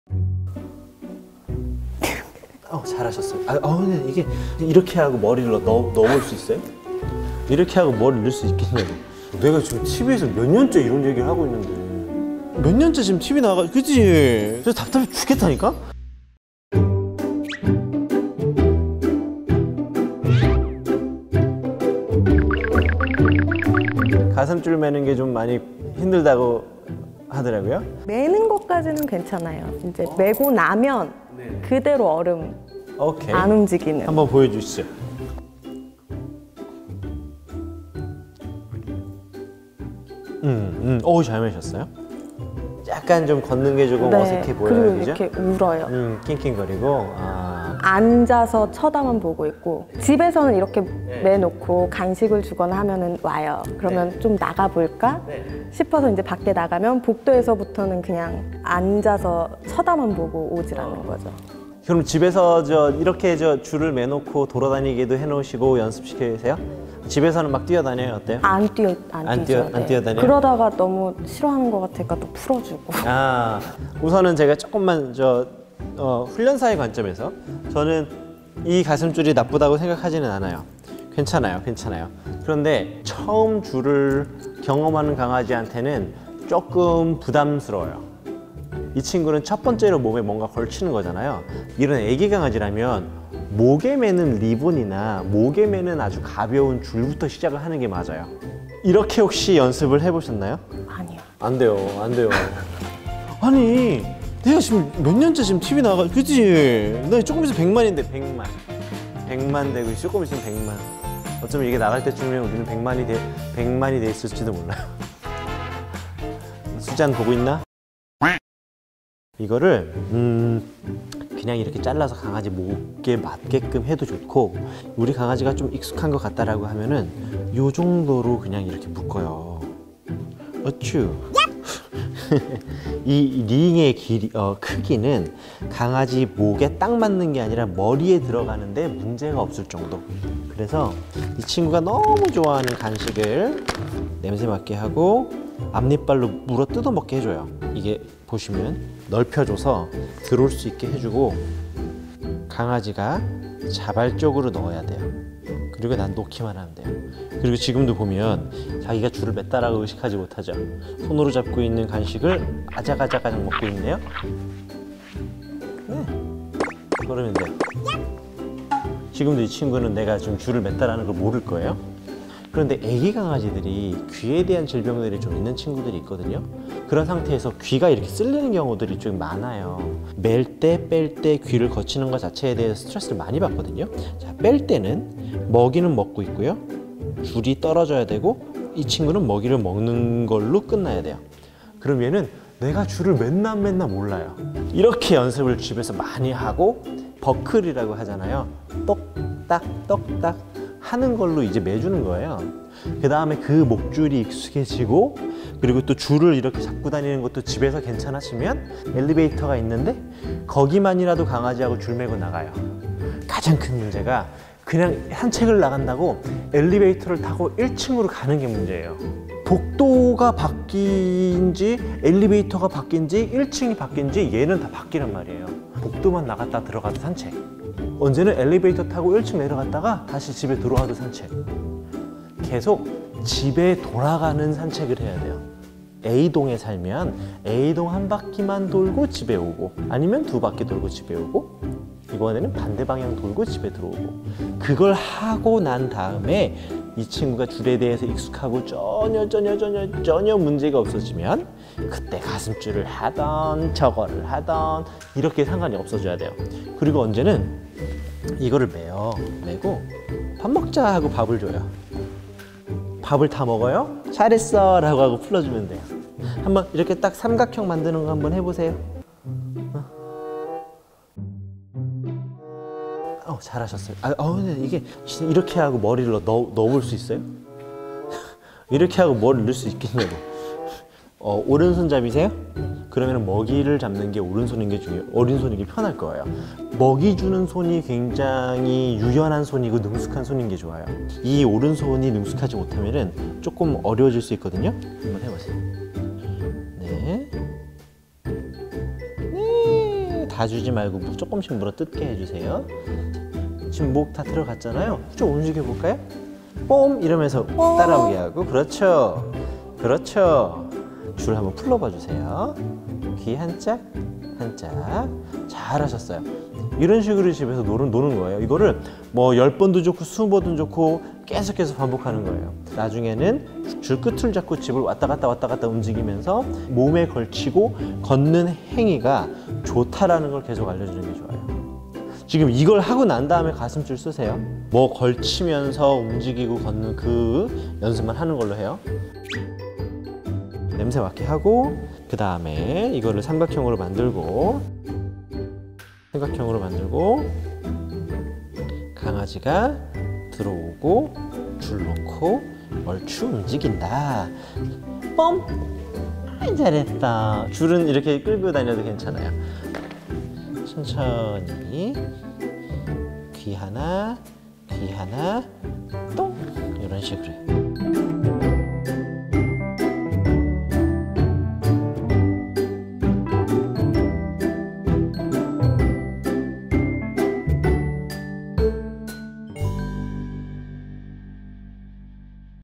가슴줄 하면 고장나는 꼬마 비숑 강아지│강형욱의 멍스쿨 초보 보호자
[Eng sub/日本語字幕]
산책은 하고 싶은데 우리 강아지는 가슴줄만 하면 고장나 버려요(?) ㅠㅠ
낑낑대고 얼어버리는 꼬마 멈뭄이들에게
꼭 필요한 이 교육은 무엇일까요?

▶퍼피에게 필요한 목줄은?

혼내지 않아도 혼나지 않아도 되는 반려견 교육, 보듬